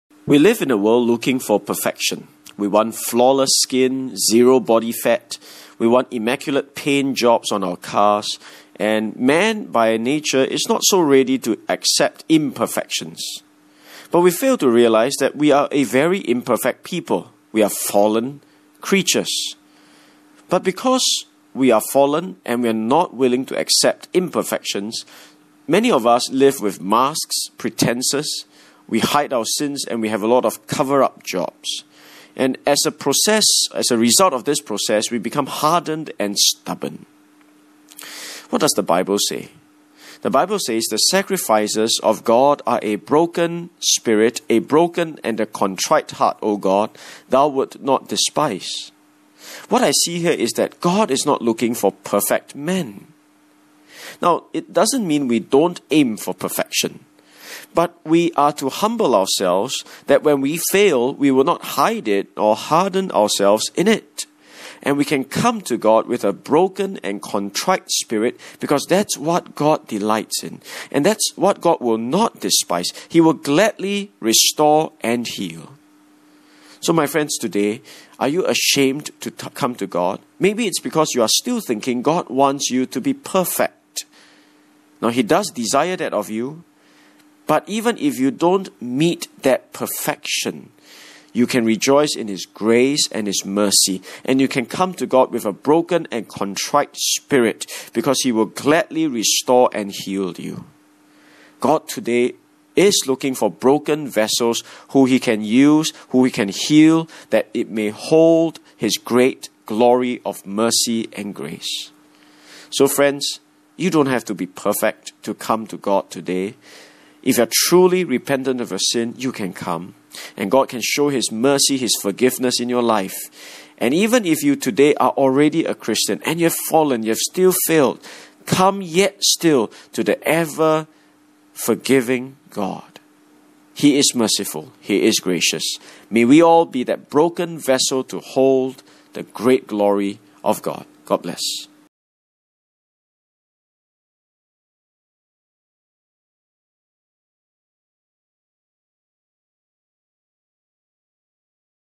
Vessels of God's Great Grace
We live in a world looking for perfection. We want flawless skin, zero body fat. We want immaculate paint jobs on our cars and Man by nature is not so ready to accept imperfections. But we fail to realize that we are a very imperfect people. We are fallen creatures! But because we are fallen and we are not willing to accept imperfections, many of us live with masks, pretences, we hide our sins and we have a lot of cover-up jobs,  and as a result of this process, we become hardened and stubborn.

What does the Bible say?  The Bible says, "the sacrifices of God are a broken spirit, a broken and a contrite heart O God  Thou would not despise." What I see here is that God is not looking for perfect men. Now, it doesn't mean we don't aim for perfection, but we are to humble ourselves that when we fail, we will not hide it nor harden ourselves in it. And we can come to God with a broken and contrite spirit because that's what God delights in. And that's what God will not despise. He will gladly restore and heal.

So my friends today are you ashamed to come to God? Maybe it's because you are still thinking God wants you to be perfect. Now He does desire that of you, but even if you don't meet that perfection,  you can rejoice in His grace and  His mercy. And you can come to God  with a broken and contrite spirit. because He will gladly restore and heal you.

God today is looking for broken vessels who He can use, who He can heal, that they may hold His great glory of mercy and grace.

So friends,  you don't have to be perfect to come to God today. If you're truly repentant of your sin, you can come and God can show His mercy His forgiveness in your life. And even if you today are already a Christian, and you've fallen , you've still failed, come yet still to that ever forgiving God. He is merciful, He is gracious. May we all be that broken vessel to hold that great glory of God. God bless.